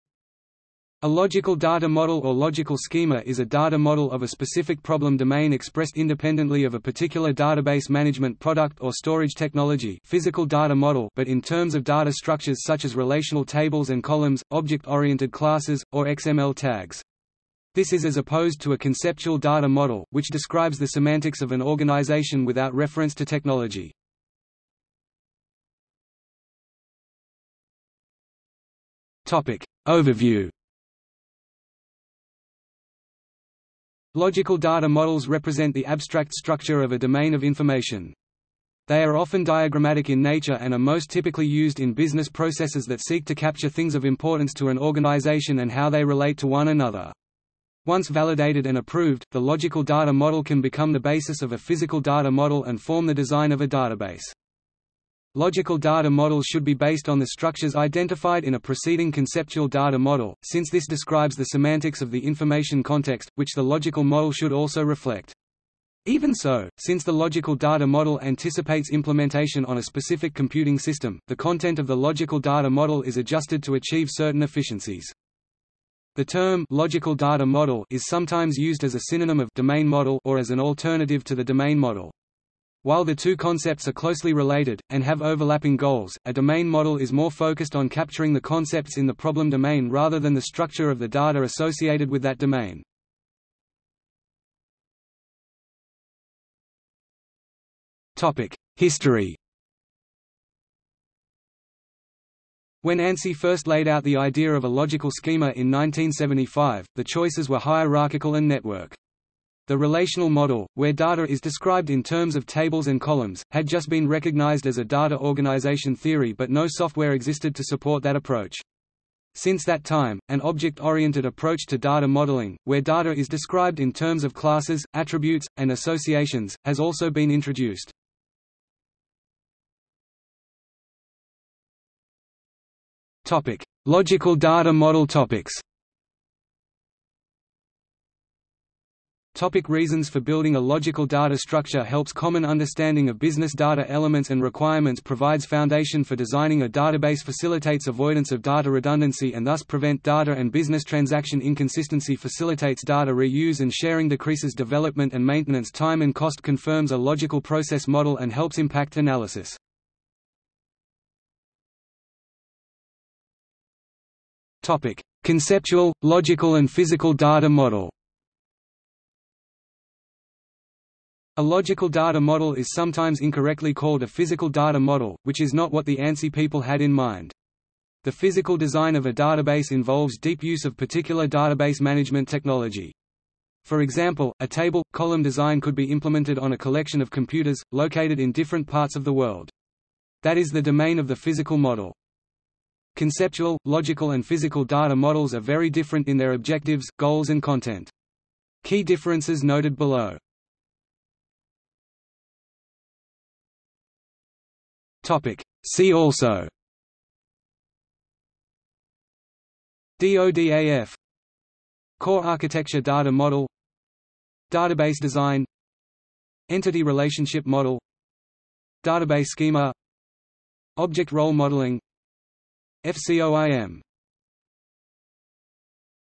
Logical schema | Wikipedia audio article
00:00:25 1 Overview
00:01:34 2 History
00:02:09 3 Logical data model topics
00:02:19 3.1 Reasons for building a logical data structure
00:02:41 3.2 Conceptual, logical and physical data model
00:03:21 4 See also

- Socrates

SUMMARY
A logical data model or logical schema is a data model of a specific problem domain expressed independently of a particular database management product or storage technology (physical data model) but in terms of data structures such as relational tables and columns, object-oriented classes, or XML tags. This is as opposed to a conceptual data model, which describes the semantics of an organization without reference to technology.